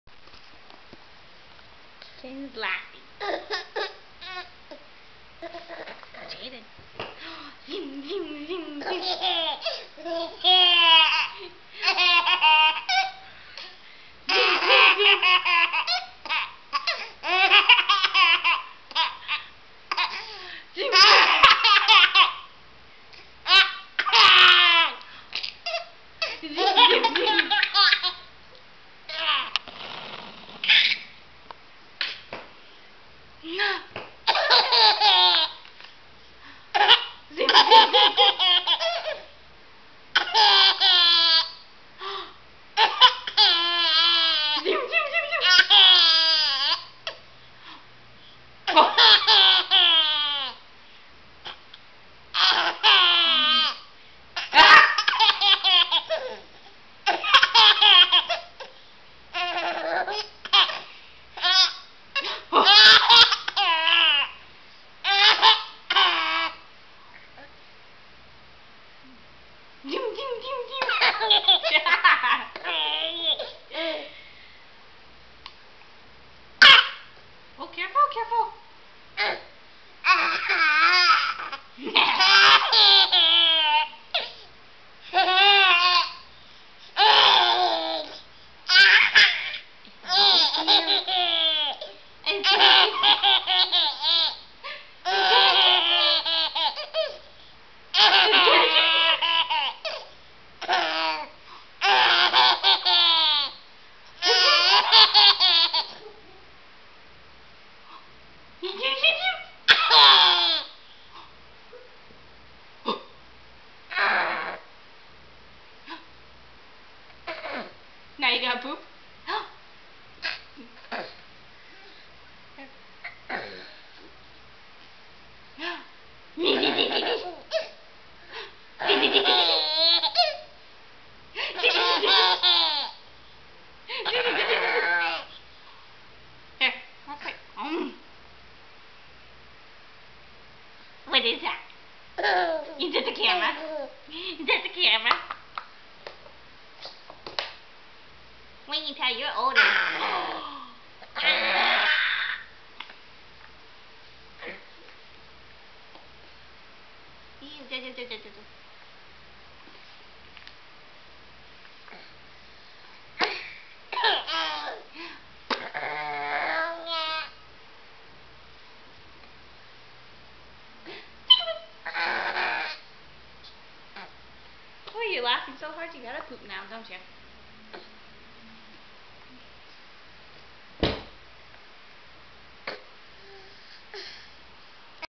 Funny kid's laugh-Funny airplane spoon :)
My son, Jayden was getting dinner feed and wasn't wanting to eat. I thought about doing the airplane spoon that I seen many mother's do before :). The first time I did it Jayden just wouldn't stop laughing! Then a few more times I thought "okay he think's this is just the funniest thing in the world!" lol and grab the camera to video tap it...the funny thing is my son is now 22 months old and he has the same laugh! :)